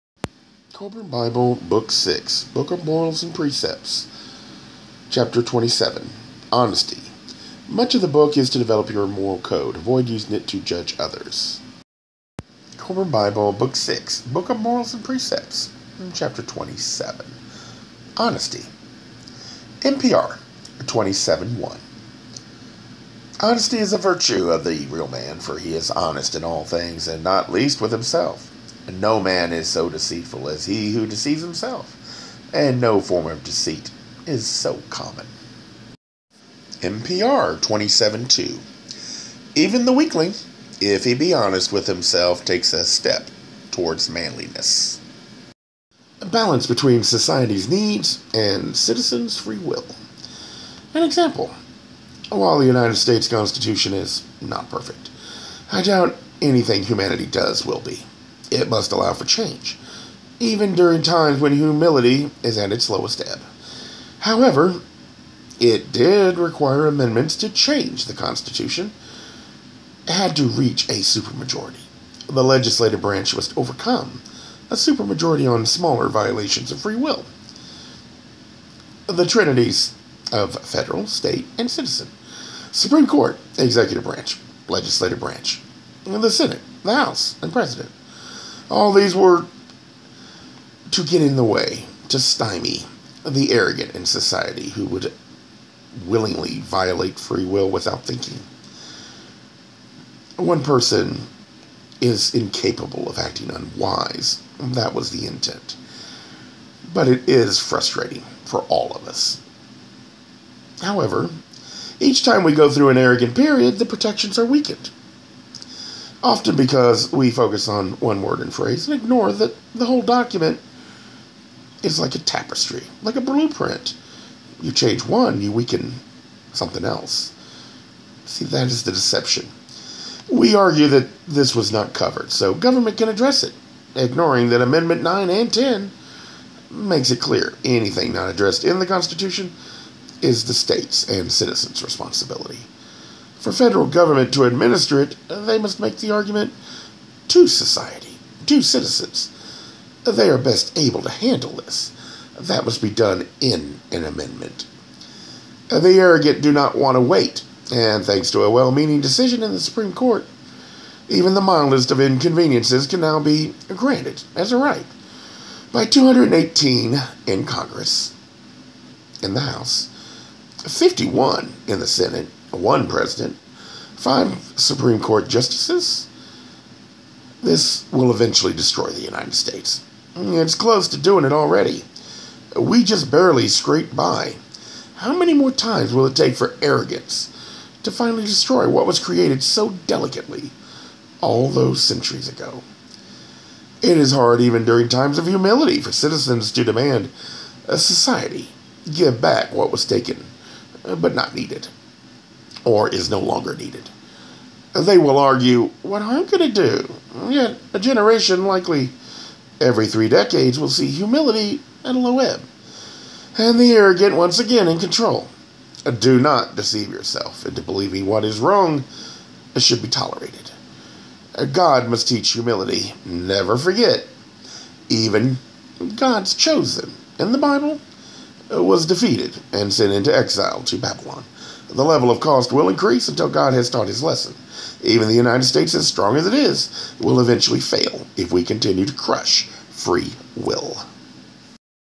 An example of balance between free will and society's needs
Comment Inspired by Kolbrin Bible Bk 6 Chp 27 honesty